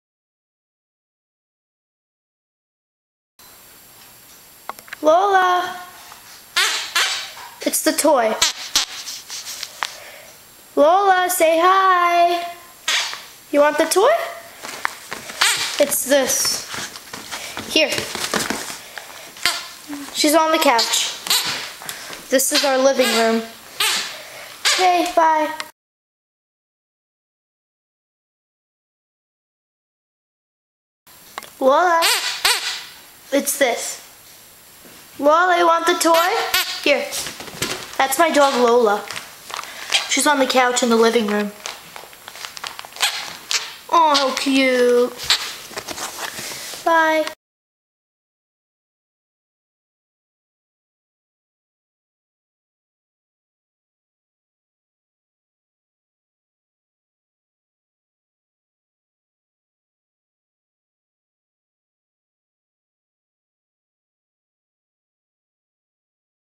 My dog Lola Playing on the couch.
This is my dog Lola. She is playing with her toy on the couch. My dog is a Yorkie Terrier.